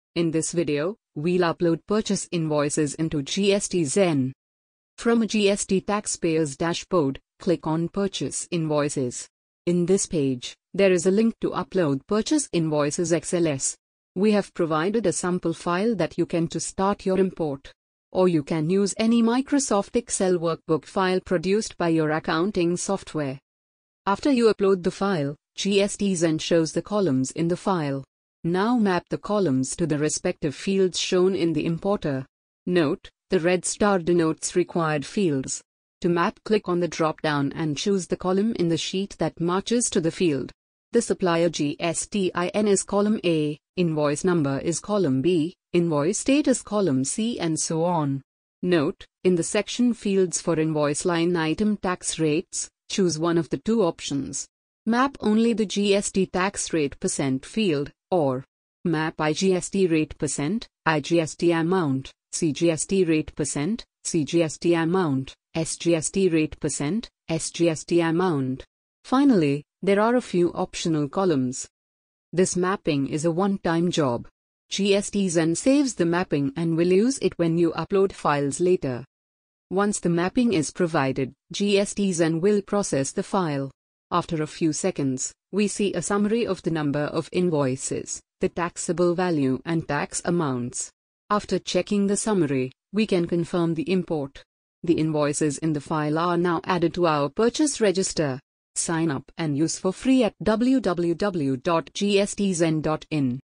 GSTZen - Bulk Upload your Purchase Register
GSTZen allows you to import your purchase data in XLSX format. You can take Excel output from any software, map the columns one time and upload Invoices and Credit Notes into GSTZen.

To know how to perform GSTR-2A vs Books reconciliation, please check